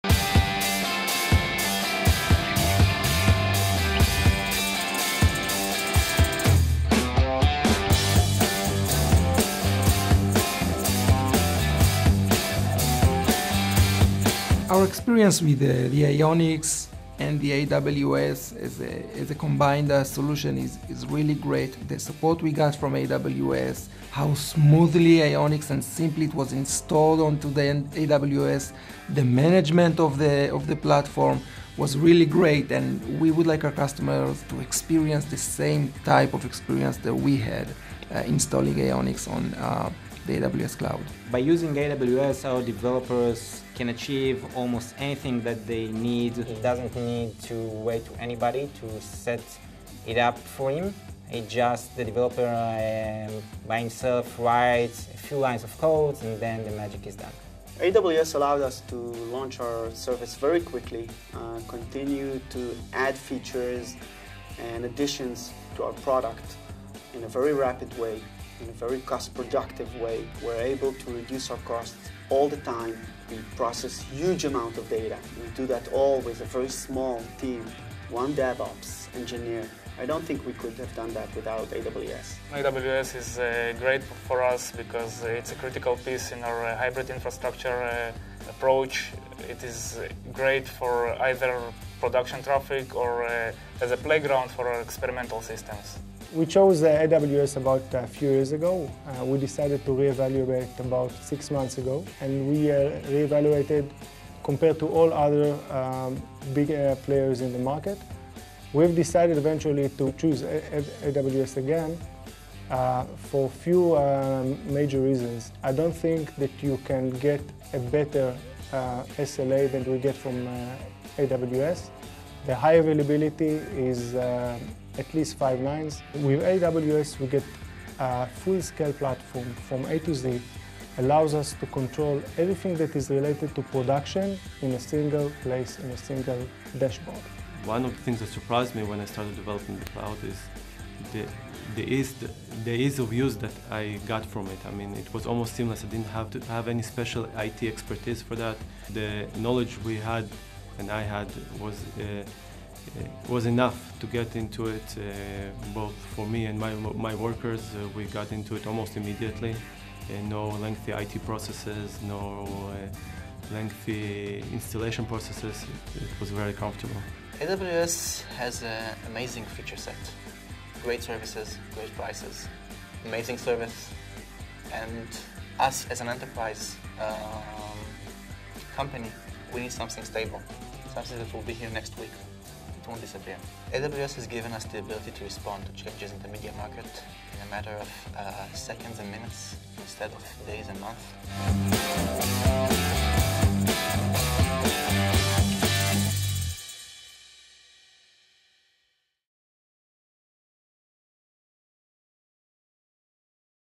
Open Video: AWS Summit | Tel Aviv
The open video that kicked off the AWS Summit | Tel Aviv, Israel in September, 2014 features customers talking about their use of the AWS Cloud. Companies featured are Magisto, YNET, MyThings, Wix, AudioCodes, Tadiran Telecom and Kenshoo.